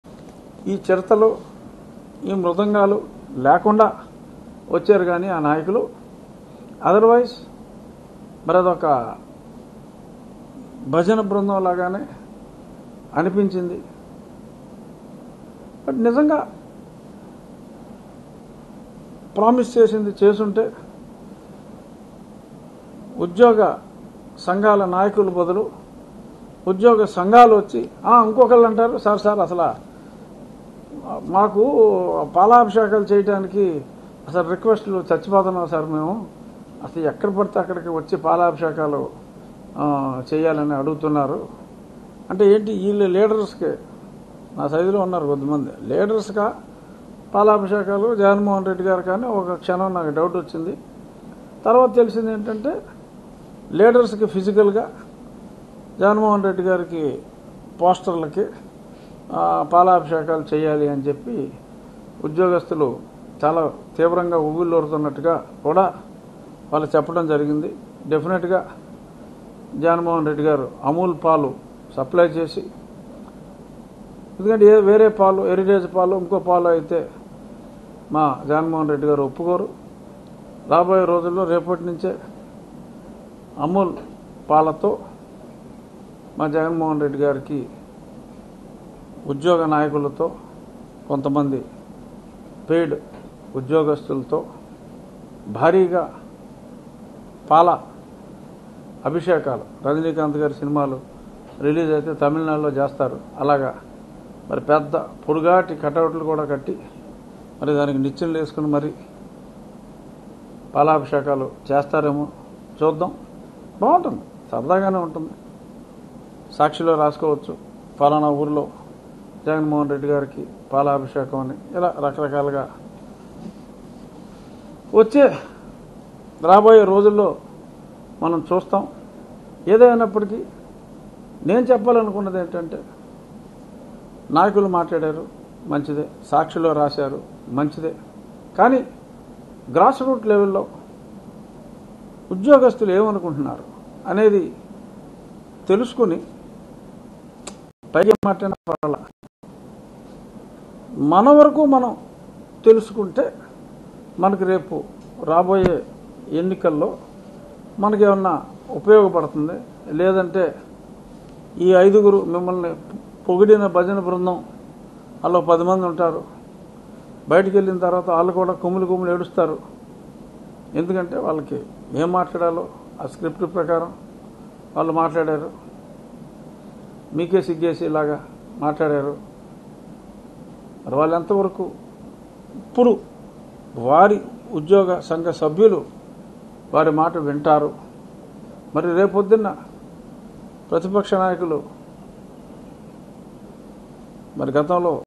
జగన్, వైసీపీ నాయకులపై రఘురామ కామెంట్స్ | Raghu Rama Raju Comments On YCP | TV5 News
LIVE : YCP MP Raghu Rama Krishnam Raju Press Meet || MP RRR Press Meet || TV5 News